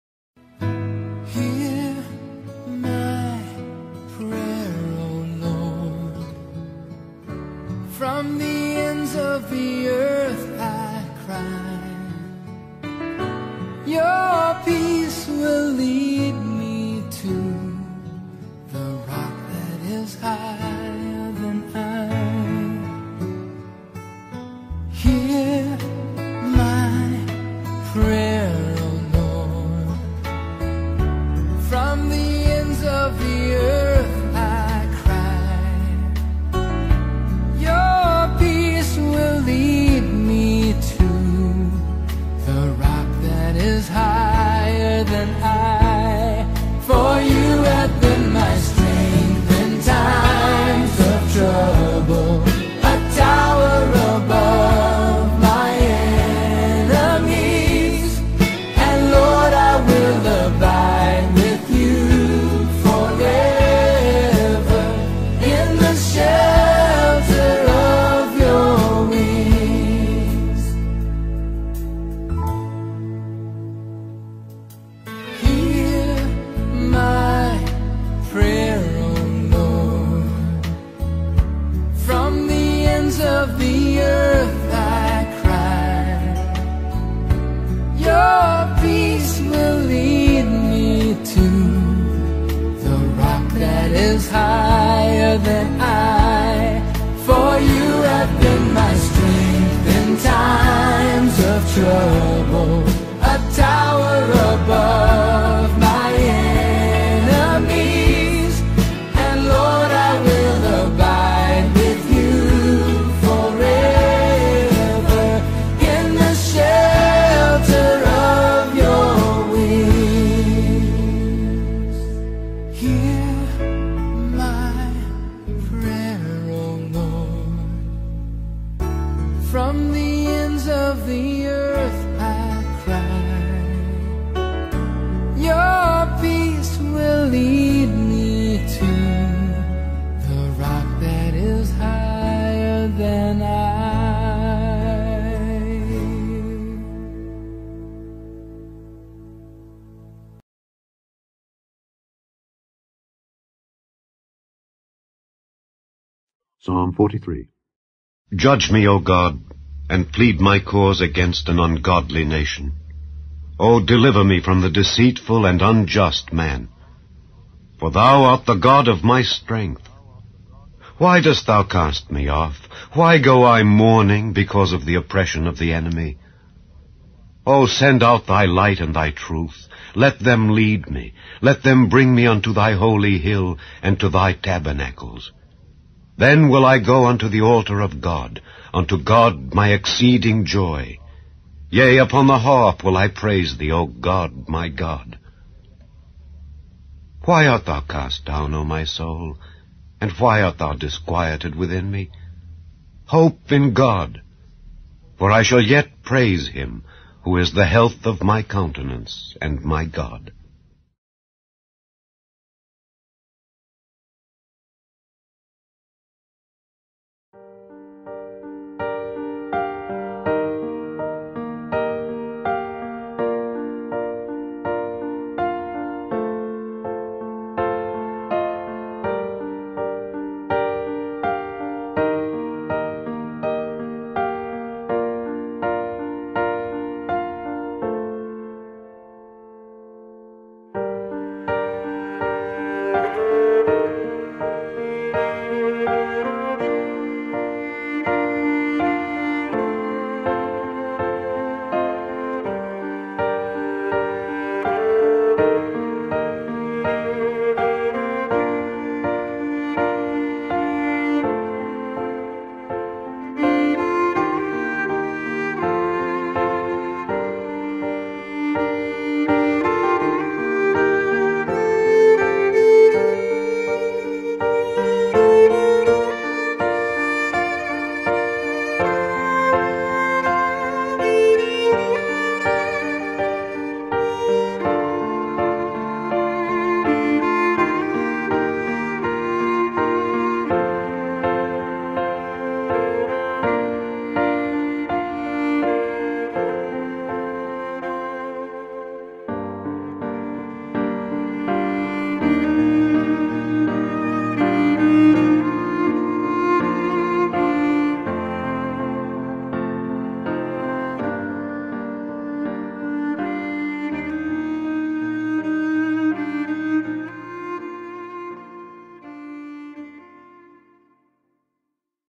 Daily Worship with Psalms (Psalms 43 - May 16, 2023)
Psalms is the 19th book of the Bible and is part of the Old Testament.

Psalms 43:
1 Judge me, O God, and plead my cause against an ungodly nation: O deliver me from the deceitful and unjust man.

2 For thou art the God of my strength: why dost thou cast me off? why go I mourning because of the oppression of the enemy?

3 O send out thy light and thy truth: let them lead me; let them bring me unto thy holy hill, and to thy tabernacles.

4 Then will I go unto the altar of God, unto God my exceeding joy: yea, upon the harp will I praise thee, O God my God.

5 Why art thou cast down, O my soul? and why art thou disquieted within me? hope in God: for I shall yet praise him, who is the health of my countenance, and my God.

Cretan Lyra: Kostas Gontikakis

Isaiah 55:11: